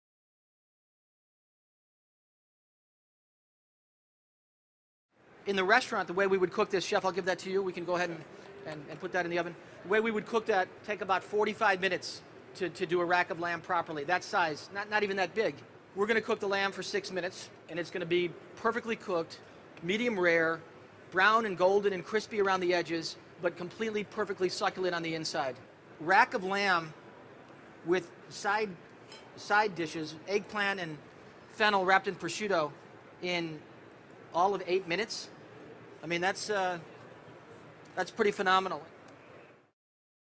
TurboChef - Rack of Lamb
Chef Charlie Trotter uses the TurboChef 30" Double Wall Speedcook Oven to roast four lamb racks in six minutes.